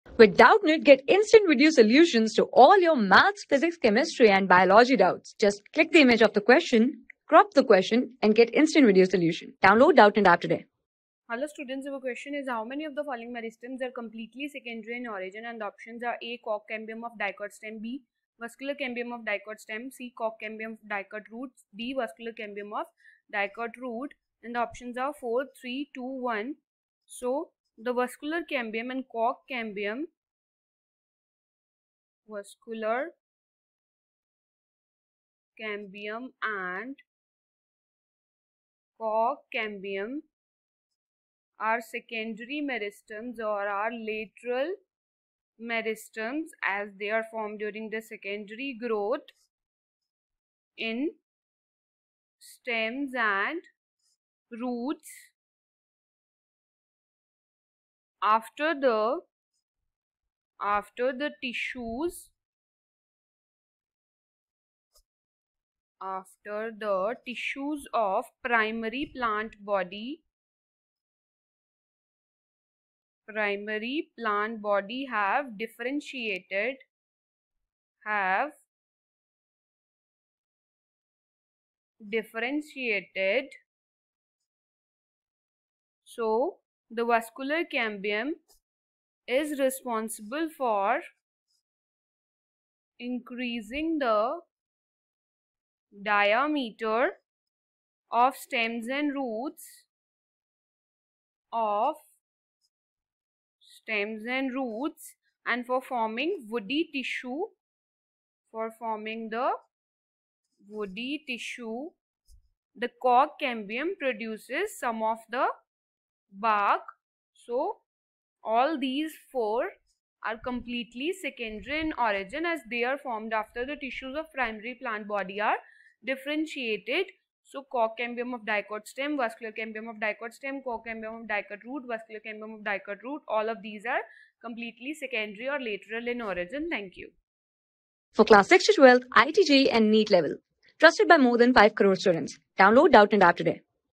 How many of the following meristems are completely secondary in origin ? A. Cork cambium of a di...
How many of the following meristems are completely secondary in origin ? A. Cork cambium of a dicot stem B. Vascular cambium of a dicot stem C. Cork cambium of a dicot root D. Vascular cambium of a dicot root
Class: 12
Subject: BIOLOGY
Chapter: NTA NEET TEST 103
Board:NEET

👉 Have Any Query? Ask Us.